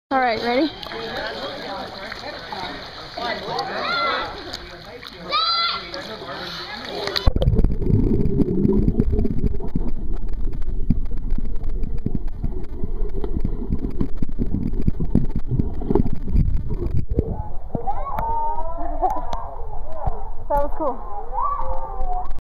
Kyle sinking to the bottom!
Kyle and me at my neighborhood pool!